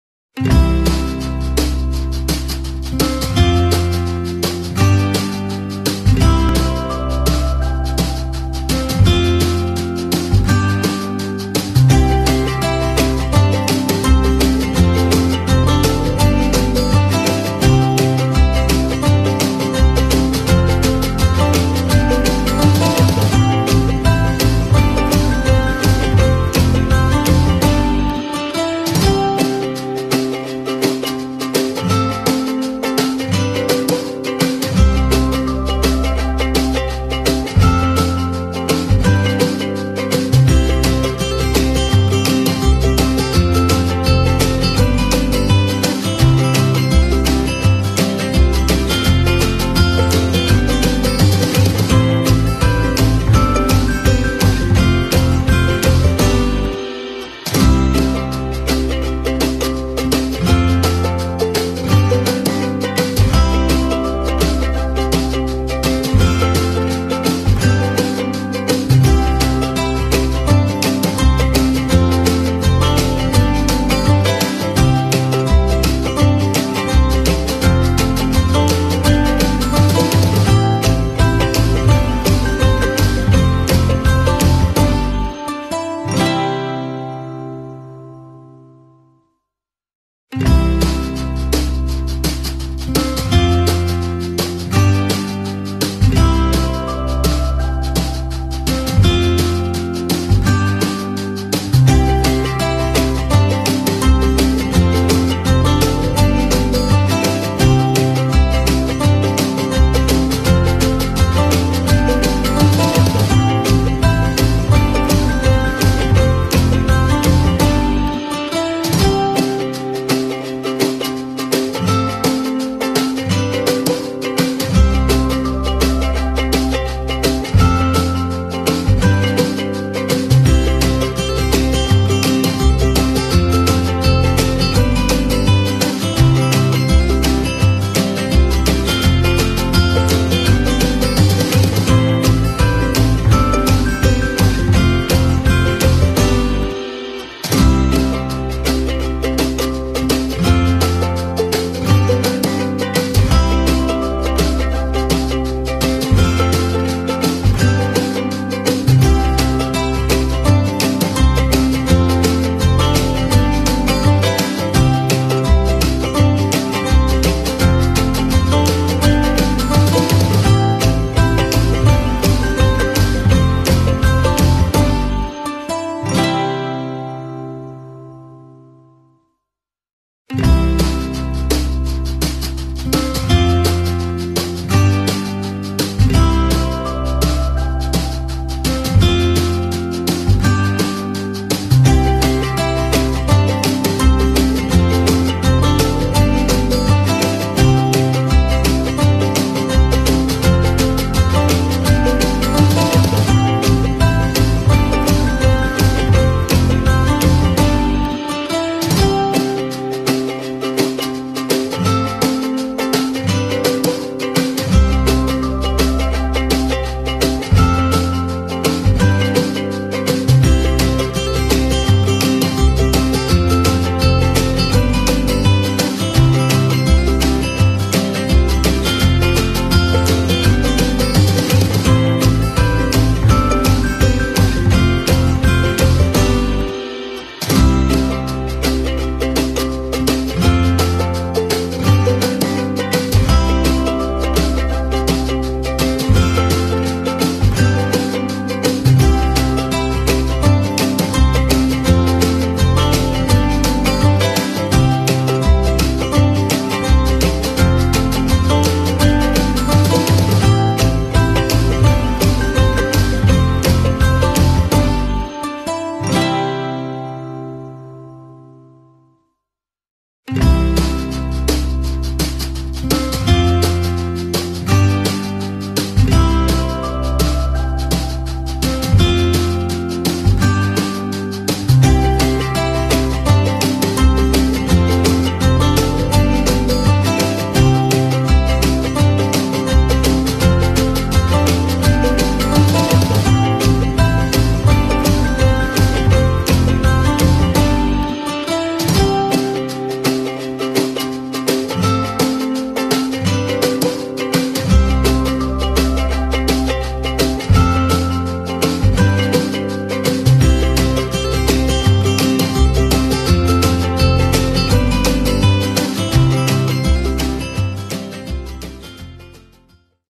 Relief Crane Loading unloading
crane is reliefign heavy load of traction motors